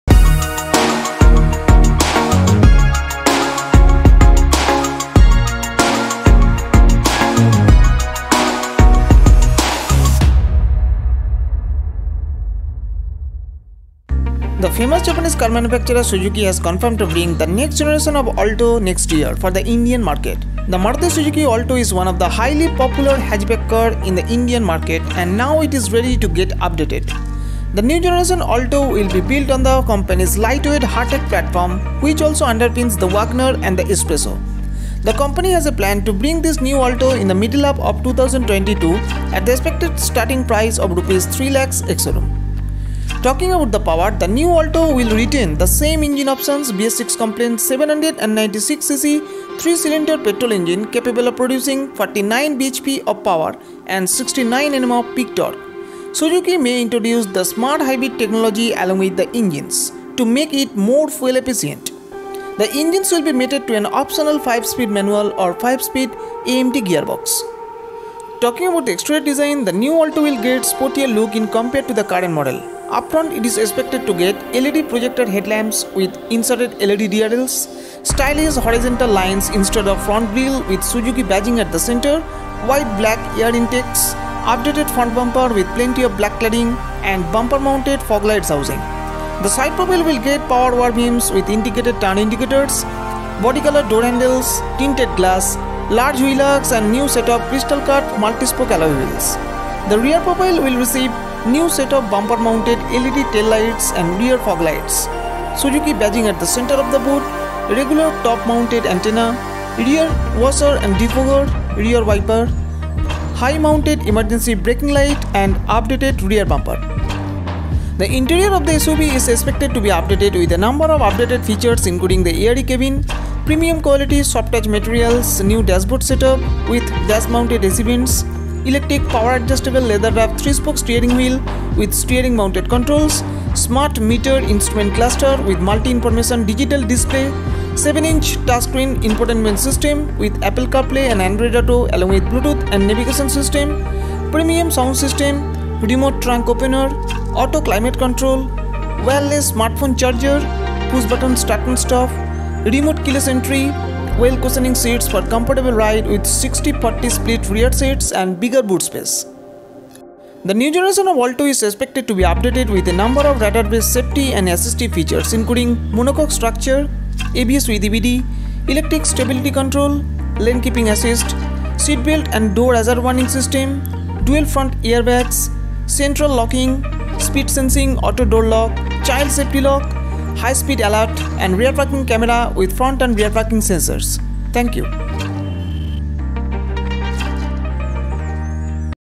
2021 Maruti Suzuki Alto 800 (Next Gen.) केबल Rs. 3 लाख में 🤩 वाह सुजुकी ने क्या गाड़ी बनाई
In this video we bring to you the new 2021 Maruti Suzuki Alto 800. It will be the combination of power and features. Know the details.